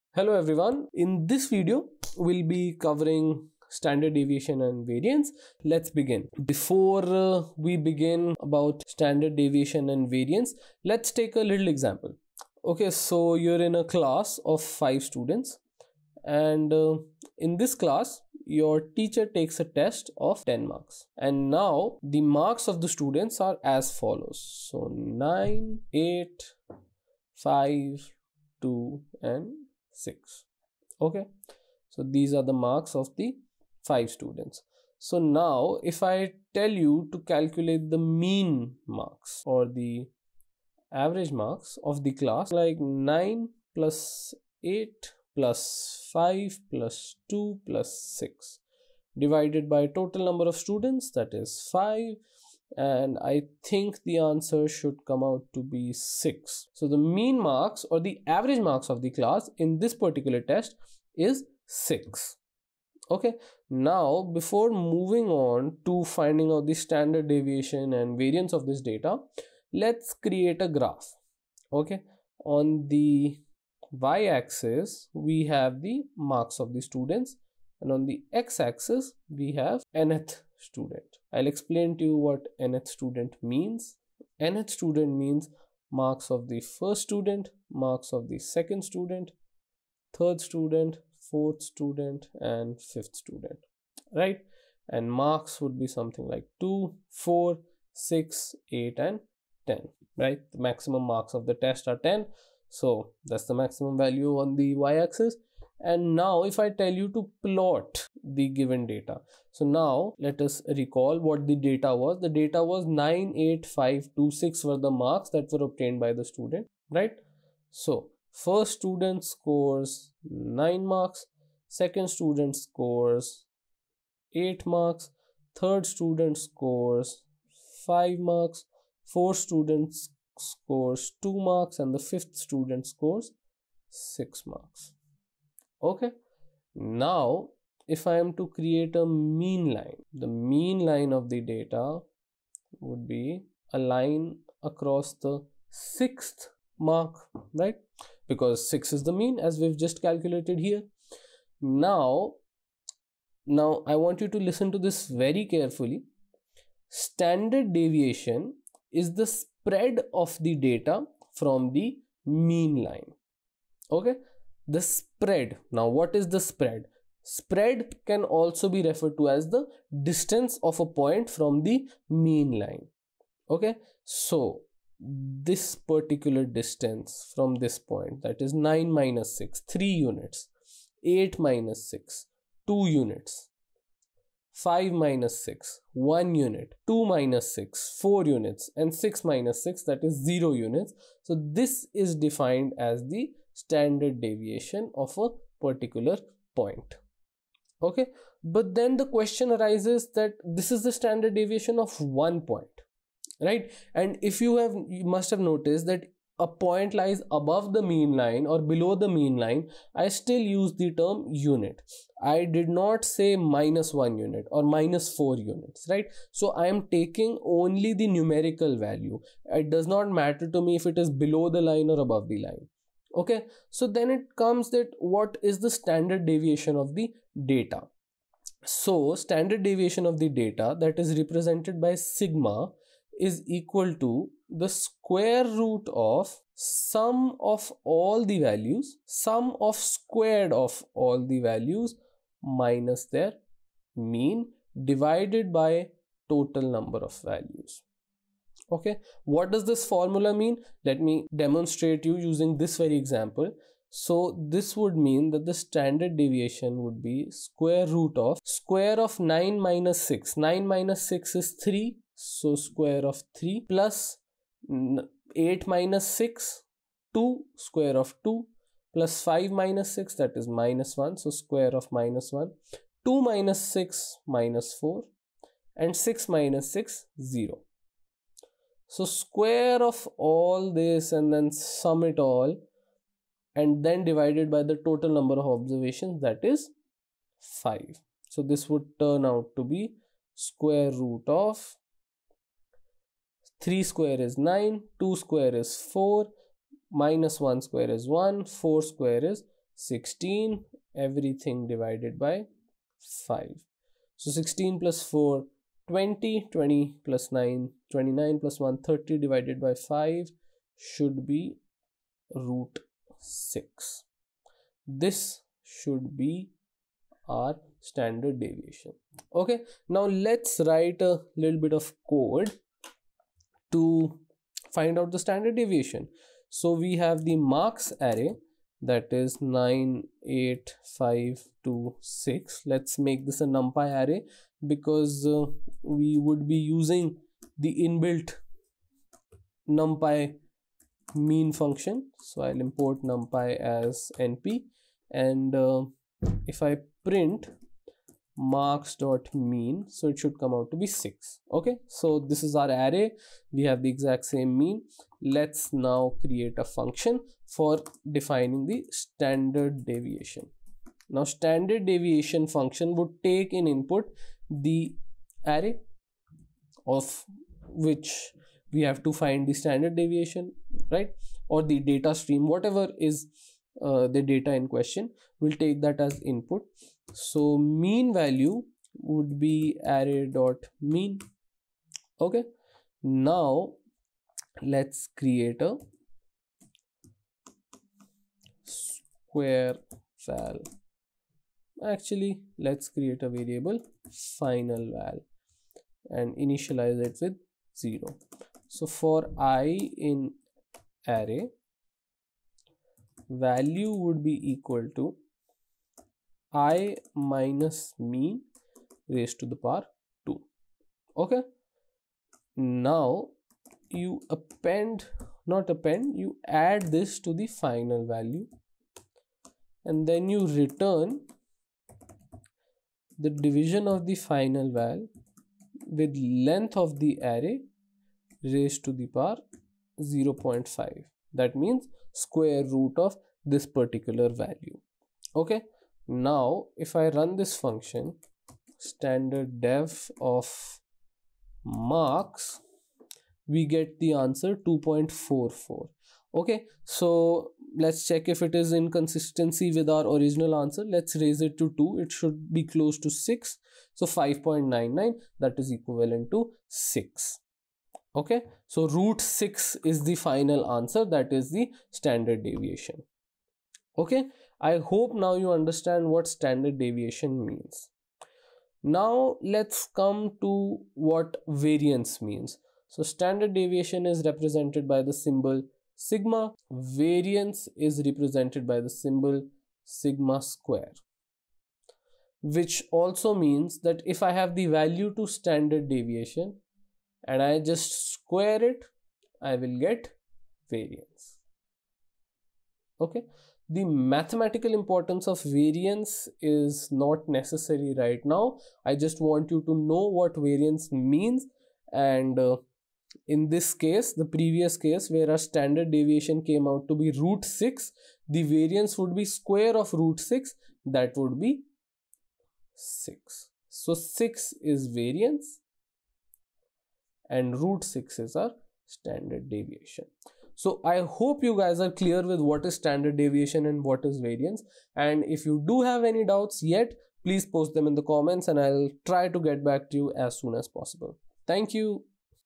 Standard Deviation and Variance | Statistics | Data Science with Python
This video explains the fundamental concepts of Standard Deviation and Variance in statistics.